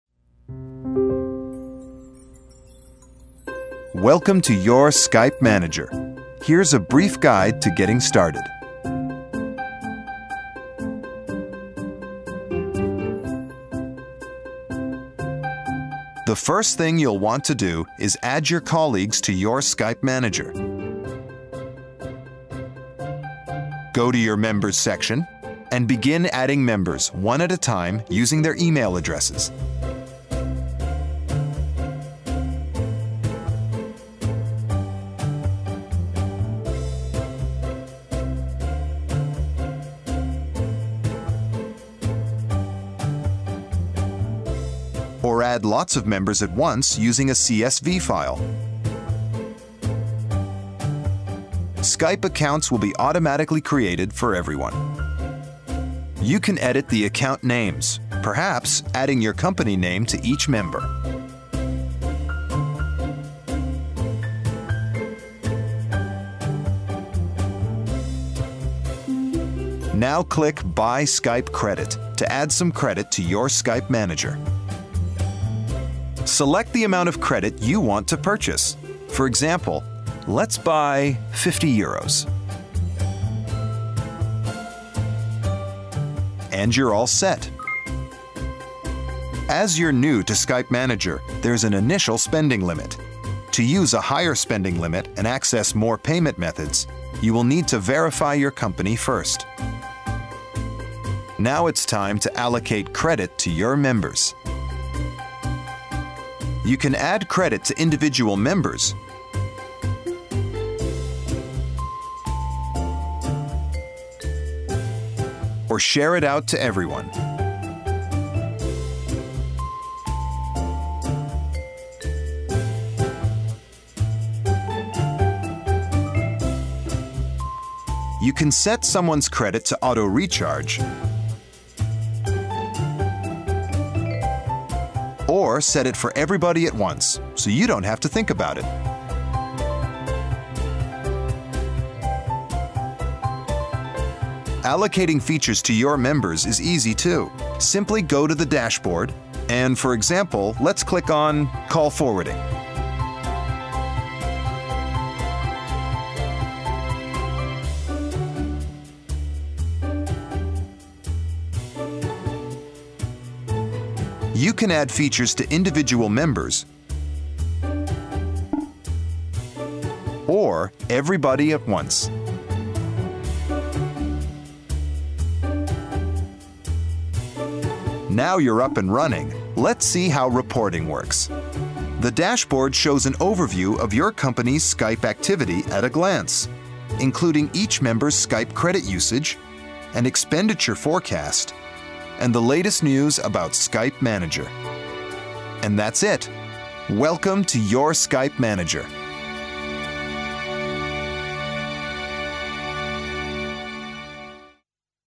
Skype Manager: Getting Started
A brief guide on how to get started with Skype Manager.

Skype Manager is a web-based management tool that lets you centrally manage Skype for businesses of any size. Try Skype Manager today and discover how easy it is to create Skype accounts for your colleagues, allocate them credit and features and keep track of what everyoneʼs spending.

Whatʼs more, everyone in your business can work smarter by using features like group video calling, voicemail and call forwarding.